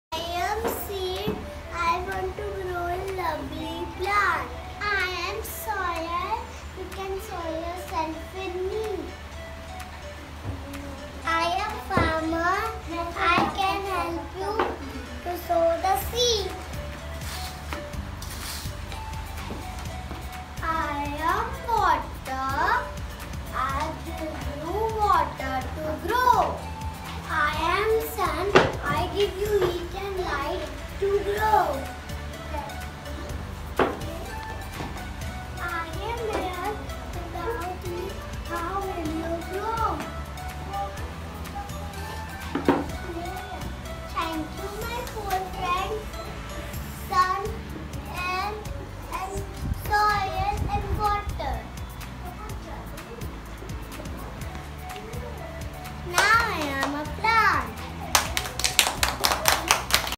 Role Play: 'Four Friends of Seeds' by KG at MRIS-51, Gurugram
"The secret of nature is patience. Patience is the first step towards learning."

Role play is an integral part of learning and acquiring concepts. As children are learning about the basic needs of a plant with the ongoing CREST theme "Wonders of Nature", Kindergartners enacted “Four Friends of a Seed”. They displayed their learning on the topic along with getting sensitized towards the need of a plant (air, water sun and soil) for growth and survival. It was a fun and educational activity.